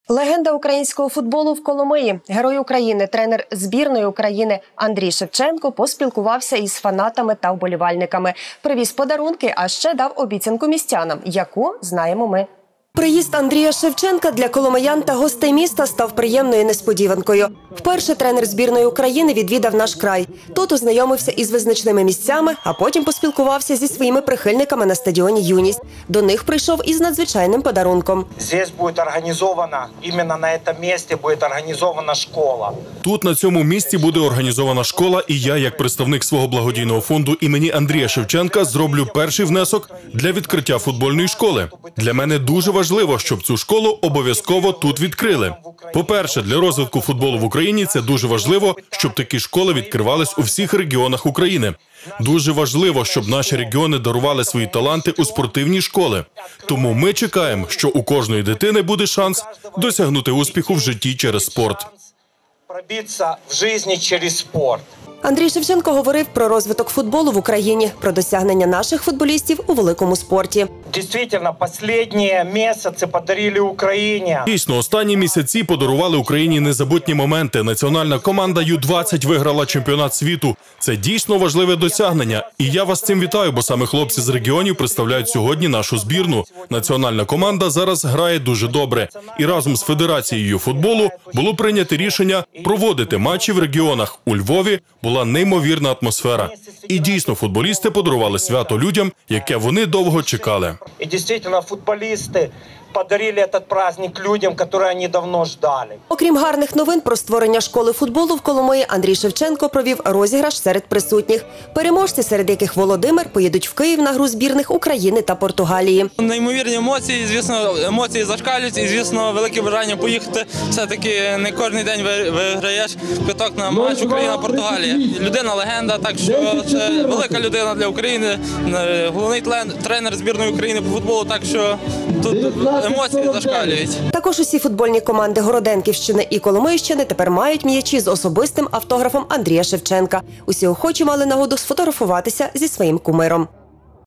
Андрій Шевченко відвідав Коломию
Легенда українського футболу – в Коломиї.

Герой України, тренер збірної України Андрій Шевченко поспілкувався з фанатами та вболівальниками.

Привіз подарунки, а ще дав обіцянку містянам. Яку - знаємо ми.